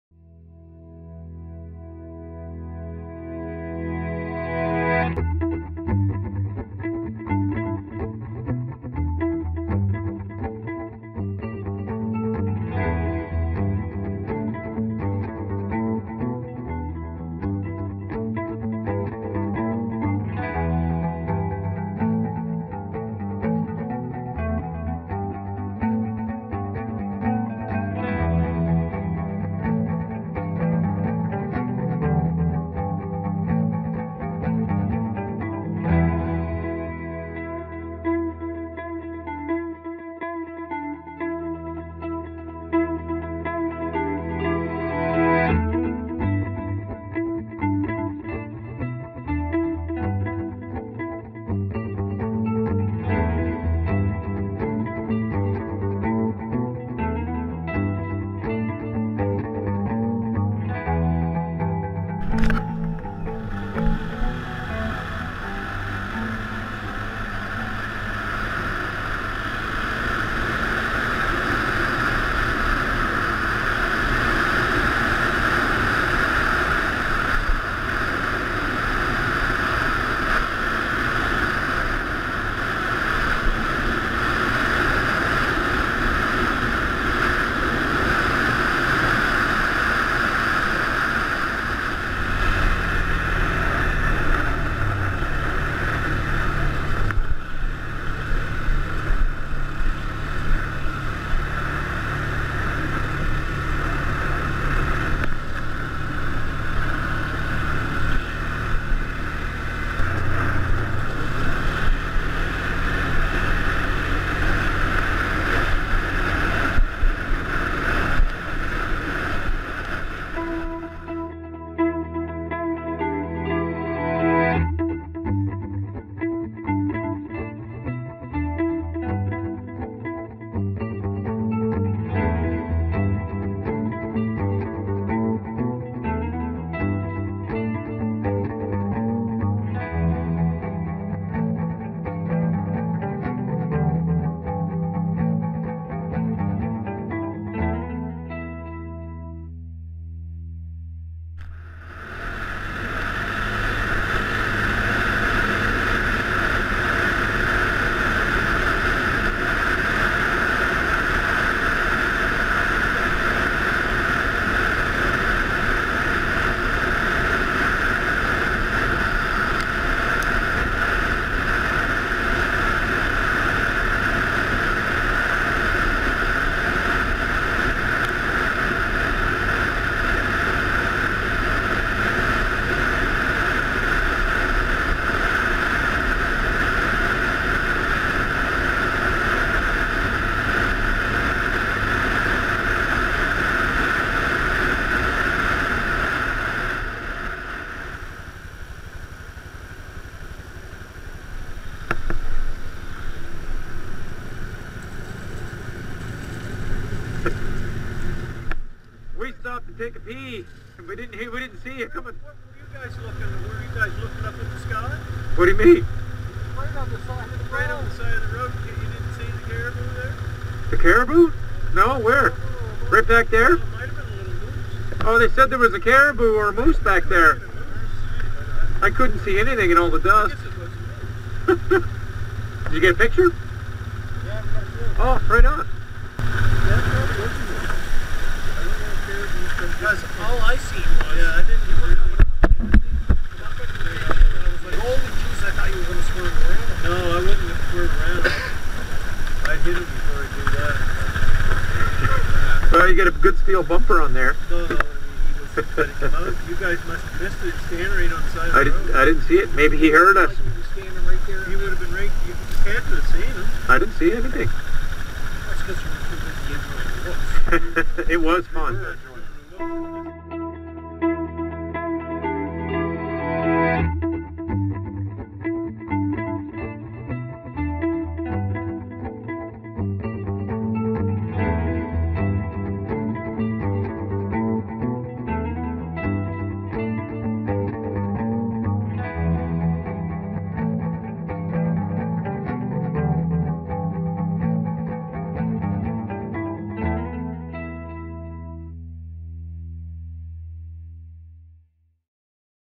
Crossing Newfoundland By ATV 2014 - Part 4 of 7
Day 4 of 7 - ATV Trip across Newfoundland - Howley to Badger.
Check out my blog for details, photos, videos and GPS Tracks.